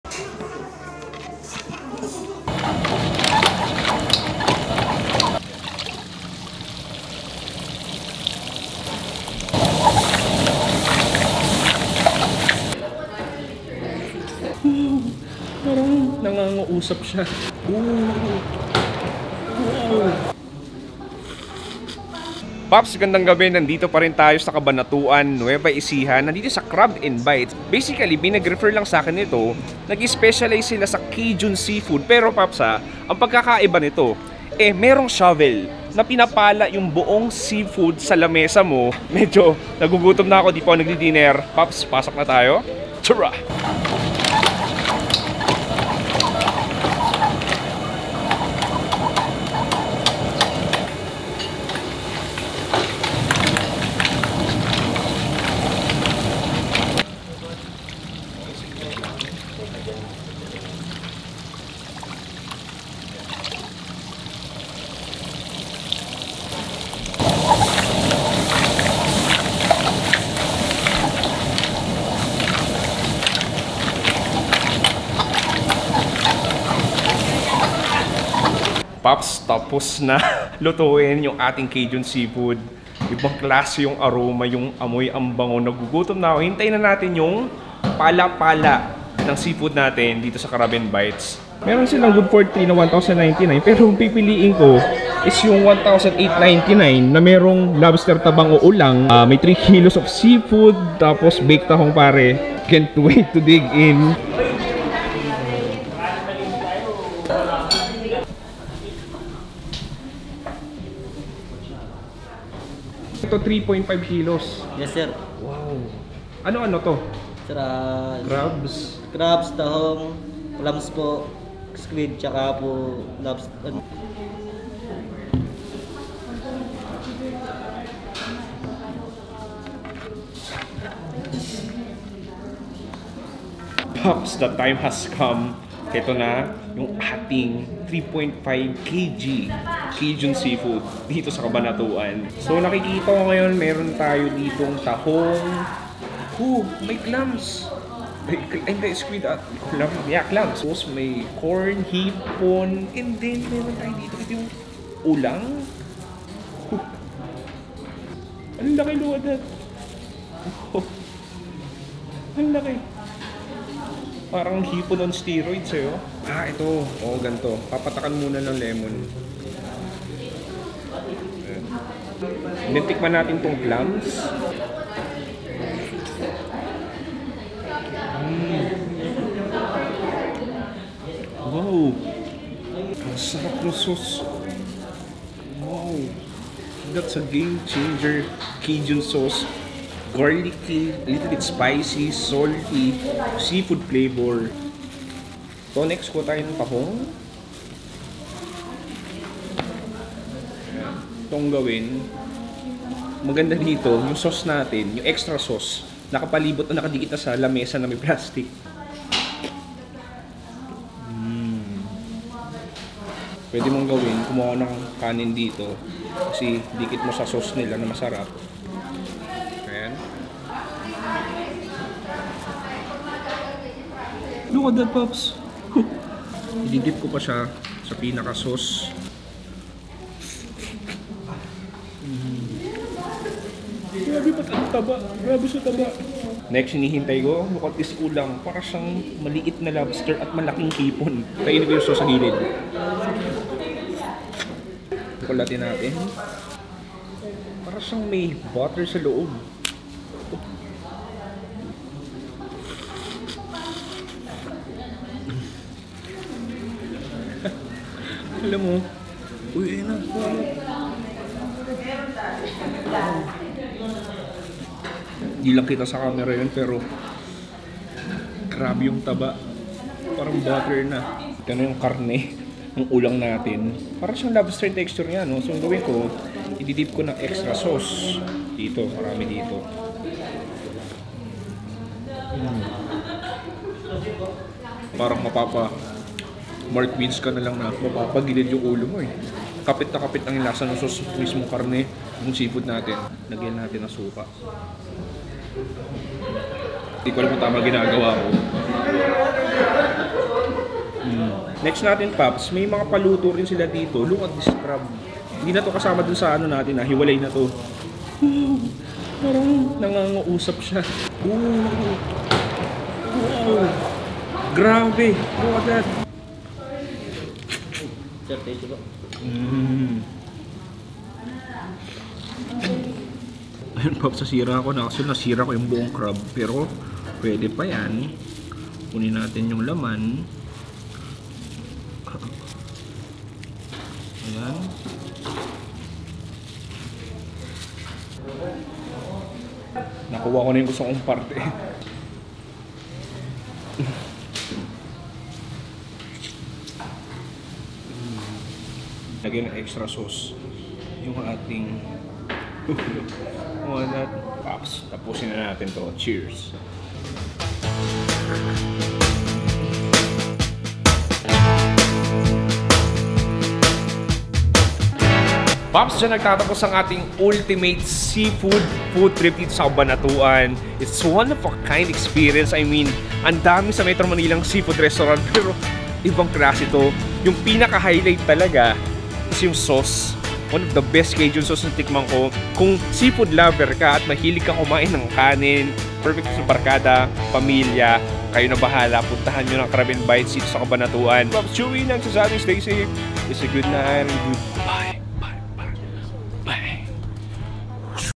3.5 KG CAJUN SEAFOOD sa Cabanatuan! Sobrang bigat, need ng SHOVEL!!
Location and Prices
📍Crabs n Bites Cabanatuan, Nueva Ecija
Prices: ₱1,800

We are 995,000 followers on Facebook!

B-Roll Camera-Carl Chuidian
Editor-Carl Chuidian
Producer-Carl Chuidian